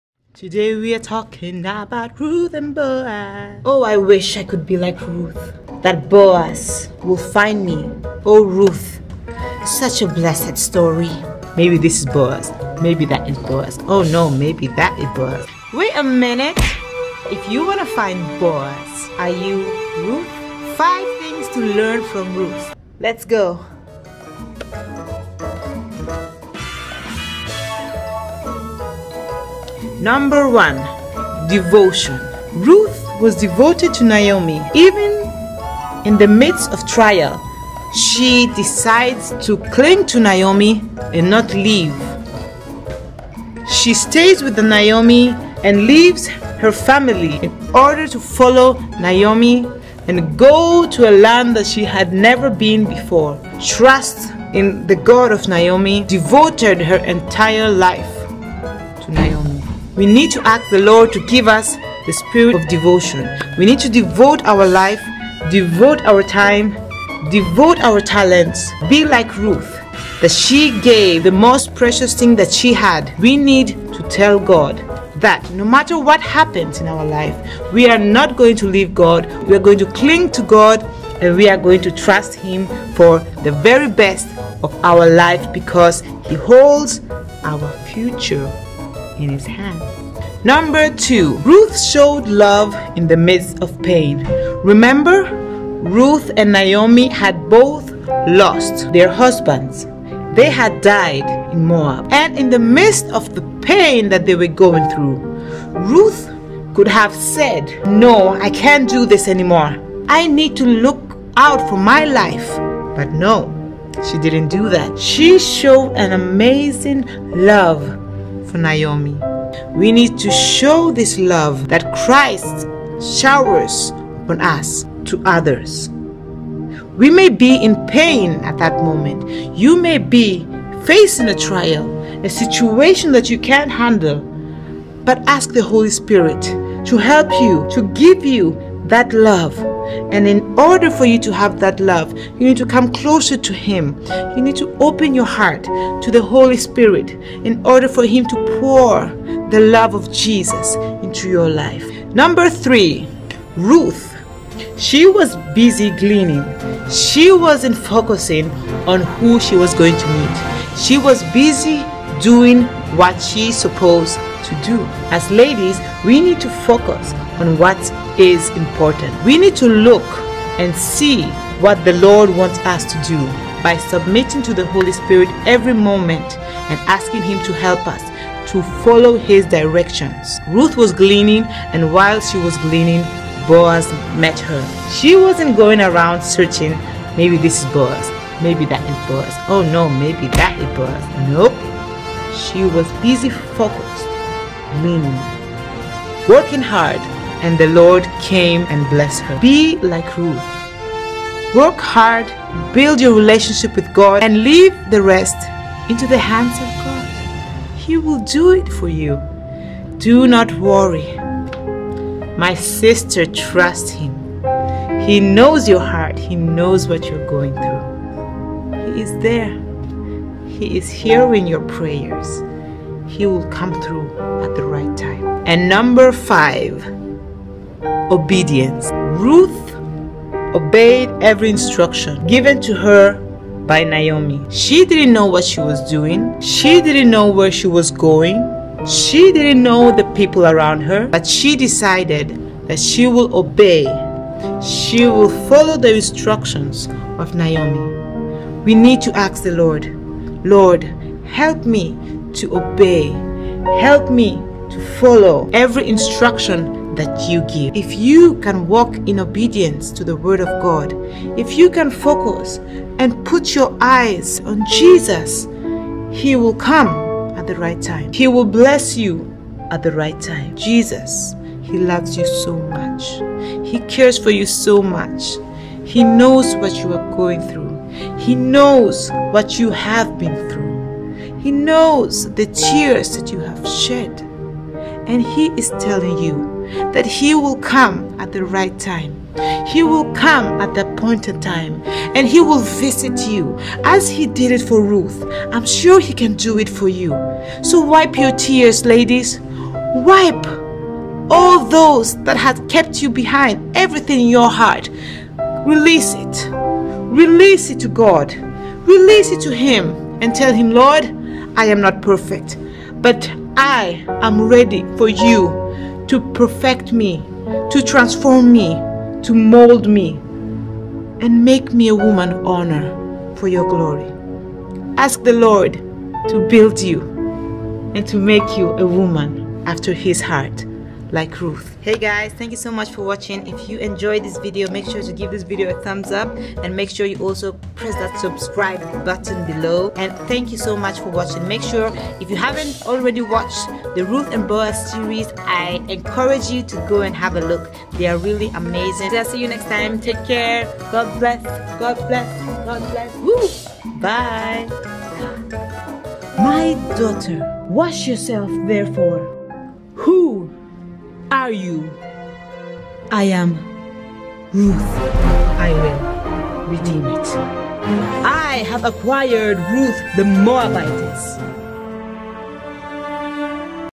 5 Inspiring Things to Learn from Ruth! Ruth and Boaz Series
Hi Guys! So here are 5 Inspiring things to Learn from Ruth! Ruth and Boaz Series was a blessing! Share with a friend and make them smile :)

//CAMERA&EDITING//
✔ Camera: Nikon Coolpix S9900

// FAQ//
✔My Hair Type: 4a,4b
✔Last Relaxer Aug 28th 2010
✔Big Chop Date: Aug 28th  2011